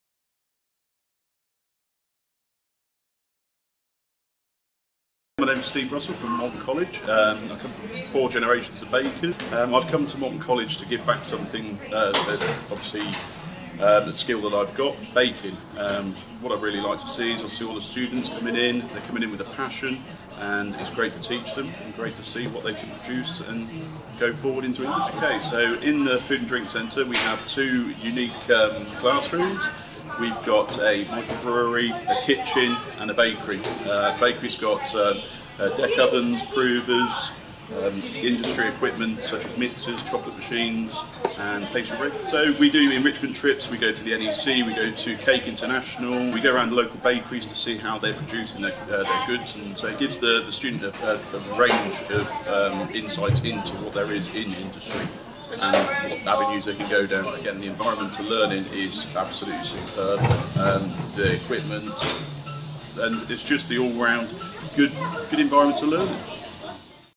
Moulton College Food and Drink Centre, with Lecturer Steve
Steve, Lecturer in Professional Bakery, takes us through why he loves to teach at Moulton College and what students can expect from the course including the fantastic facilities at Moulton.